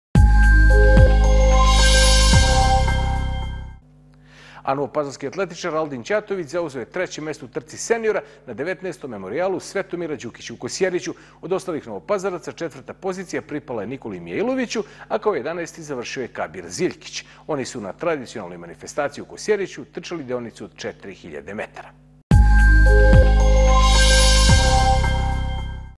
Aldin Ćatović treći u Kosjeriću
Novopazarski atletičar Aldin Ćatović zauzeo je treće mesto u trci seniora na 19. Memorijalu Svetomira Đukića u Kosjeriću. Od ostalih Novopazaraca, četvrta pozicija pripala je Nikoli Mijailoviću, a kao 11. završio je Kabir Ziljkić. Oni su na tradicionalnoj manifestaciji u Kosjeriću trčali deonicu od 4.000 metara.